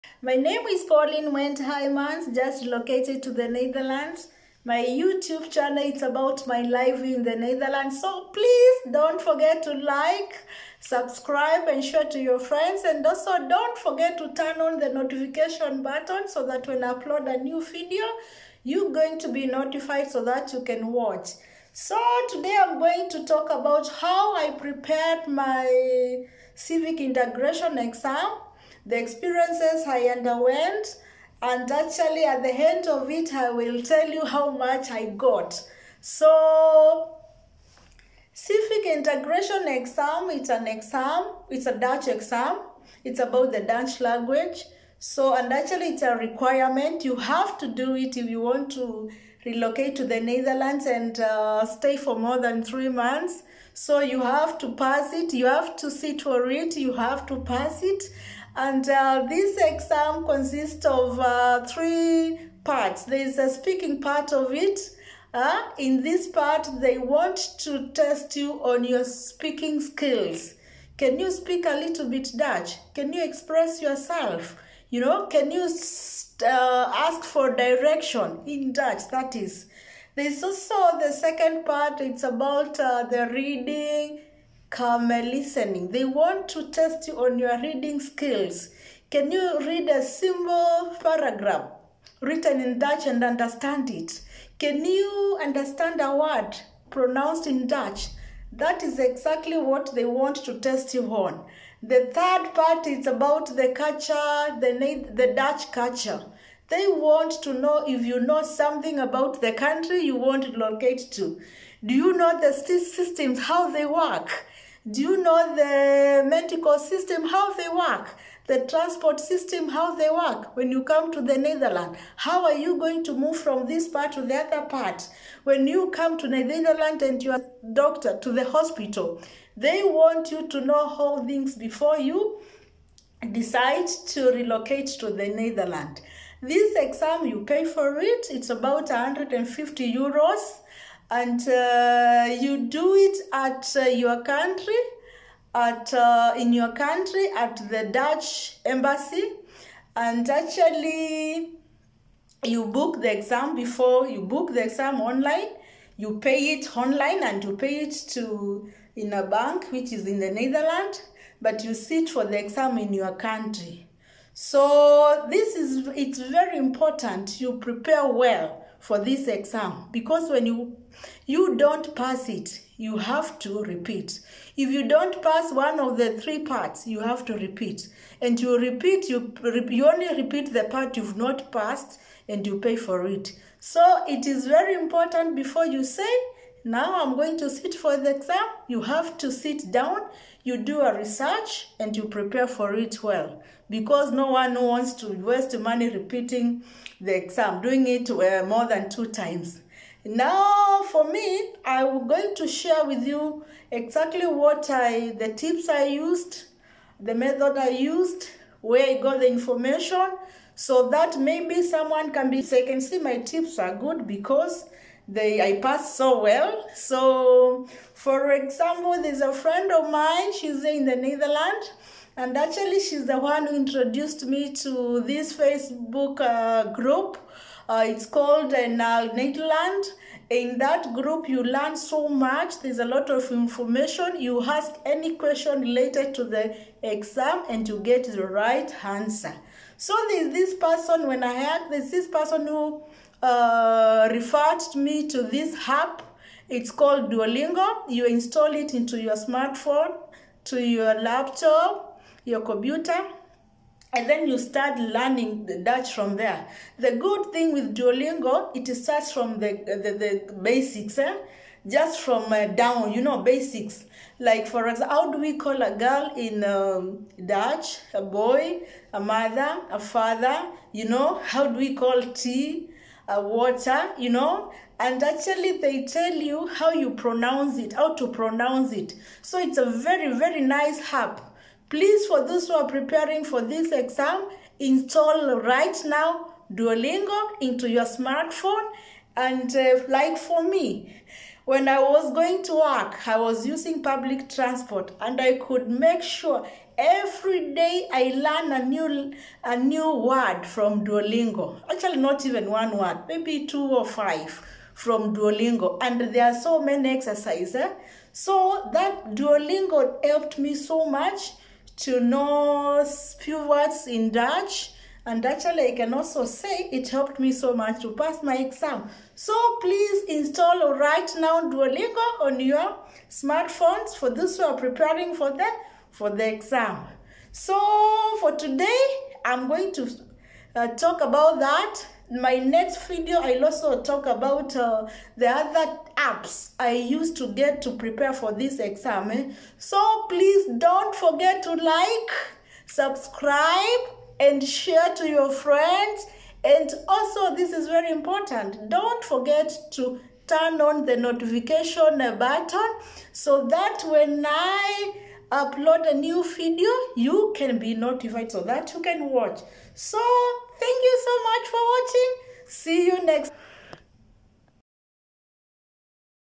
CIVIC INTEGRATION EXAMINATION (PART ONE)❤
Basisiburgering examen(civic basic exam) is a Dutch language exam which is requirement for acquiring MVV.